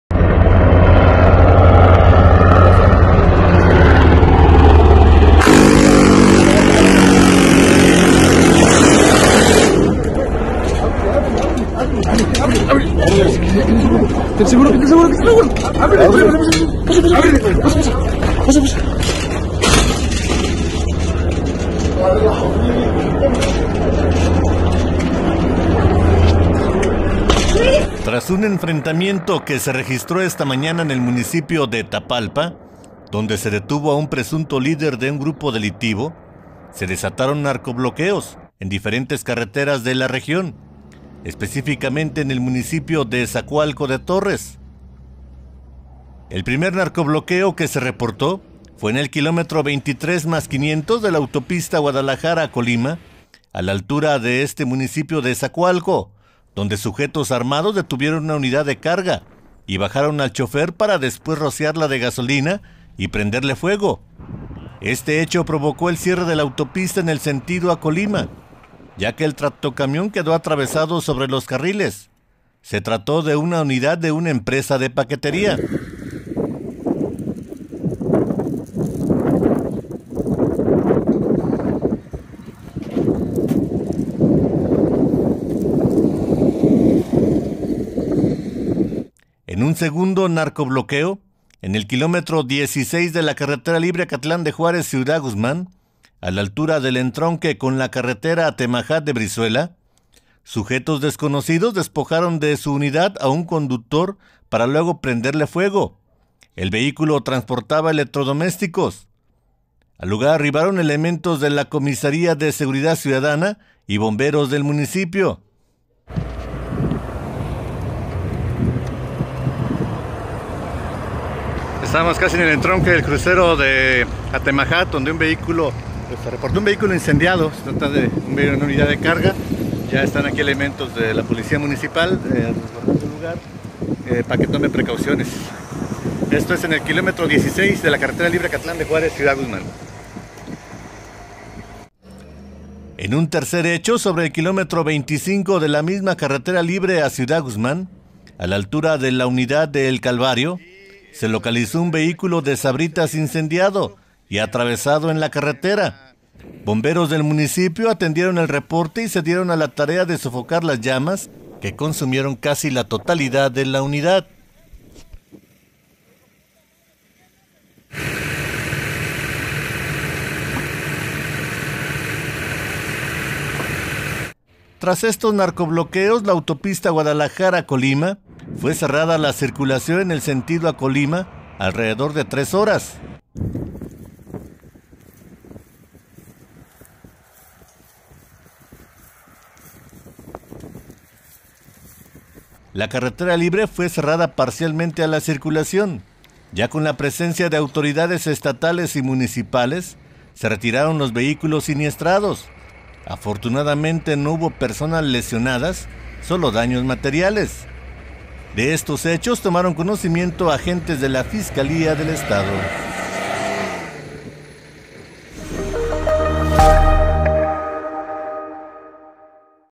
Bloqueos carreteros en Zacoalco de Torres
Tras un enfrentamiento que se registró esta mañana en el municipio de Tapalpa, donde se detuvo a un presunto líder de un grupo delictivo, se desataron narcobloqueos, en diferentes carreteras de la región, específicamente en Zacoalco de Torres.